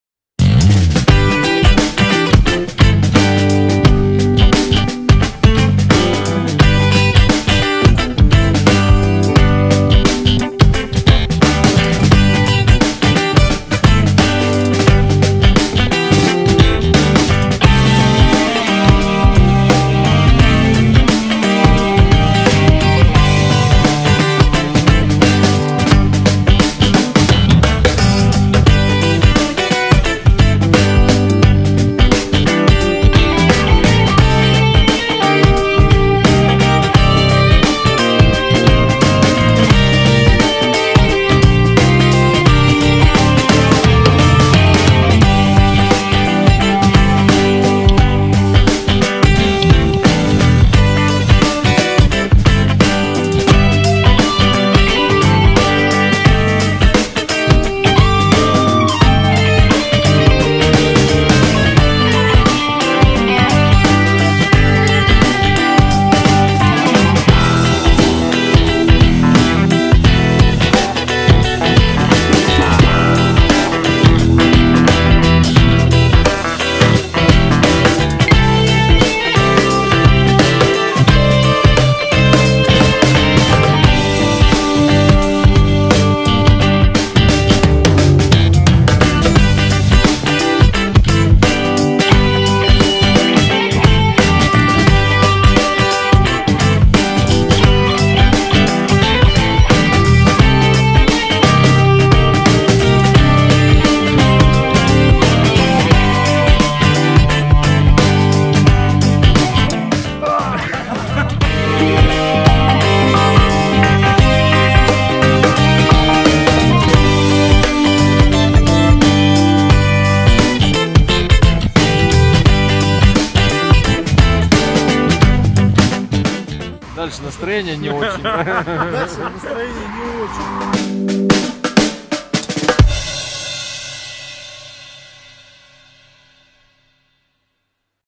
В Грузию и Турцию на Африках (2018г). Трейлер
Из Челябинска в Грузию и в Турцию на мотоциклах Хонда Африка Твин в мае 2018 г.. Фильм состоит из 5 частей.
Участники: Виталий Соболев, Дмитрий Соболев, Алексей Перкокуев.